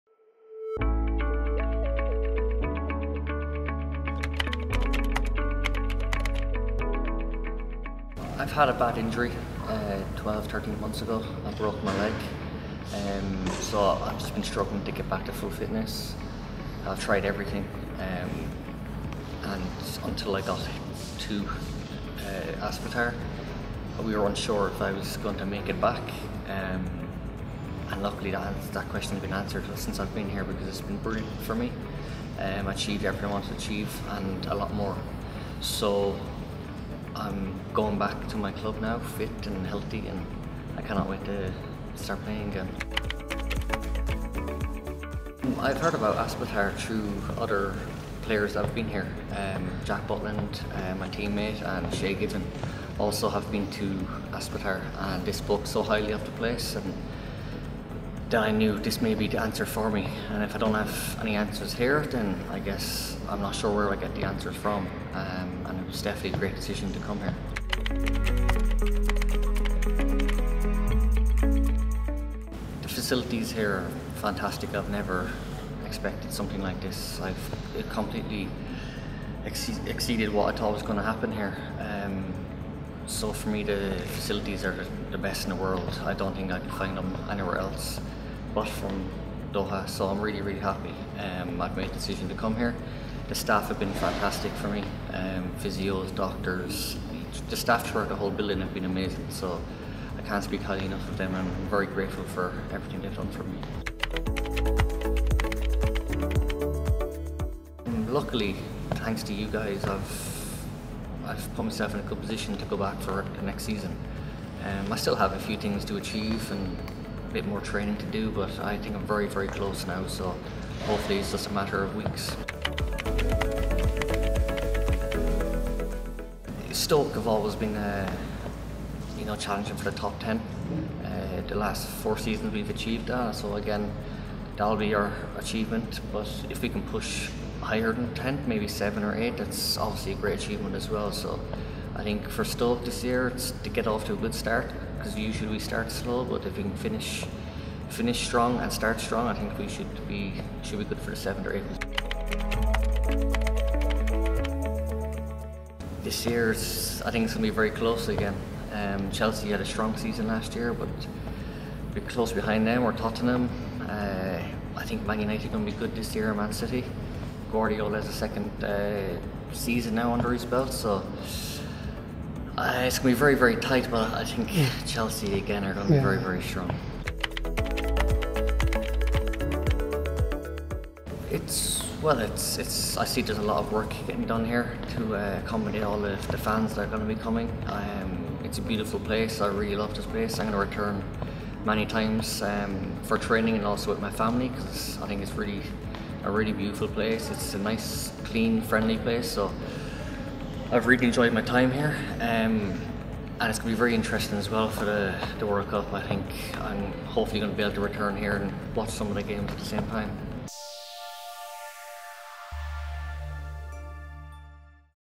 Stars At Aspetar - Footballer Stephen Ireland
Doubting his comeback after serious injury, footballer Stephen Ireland achieved more than ever expected at Aspetar.